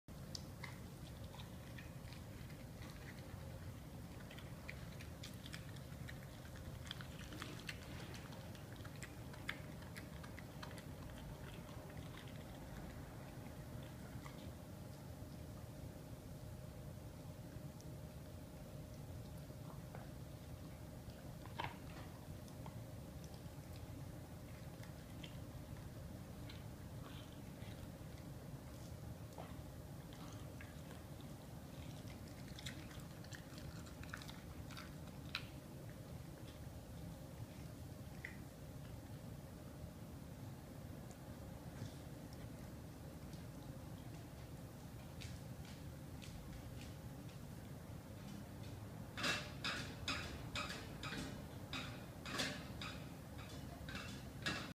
Pitbull eating raw bull penis
This is Cali, She has been raw fed since 6 weeks old.